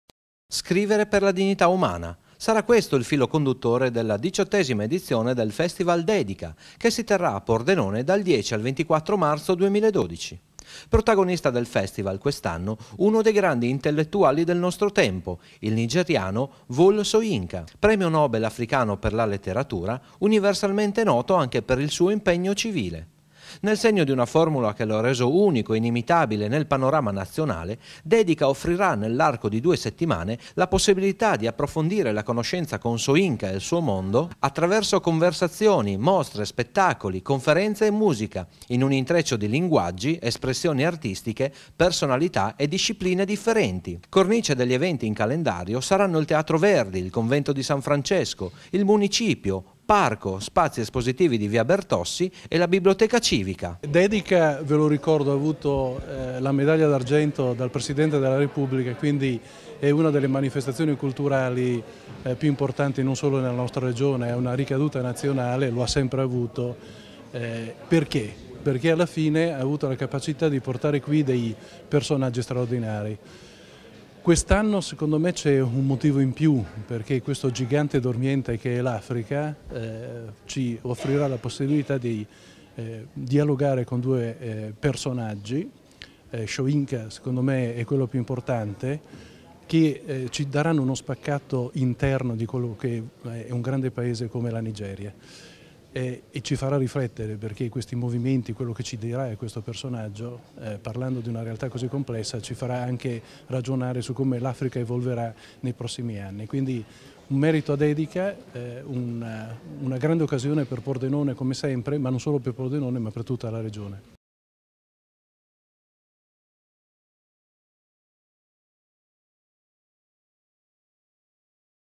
Presentazione "Dedica Festival" 2012 a Pordenone
"Scrivere per la dignità umana": sarà questo il filo conduttore della 18° edizione del festival DEDICA, che si terrà a Pordenone dal 10 al 24 marzo 2012. Protagonista del festival quest'anno uno dei grandi intellettuali del nostro tempo: il nigeriano Wole Soyinka, primo Nobel africano per la letteratura, universalmente noto anche per il suo impegno civile.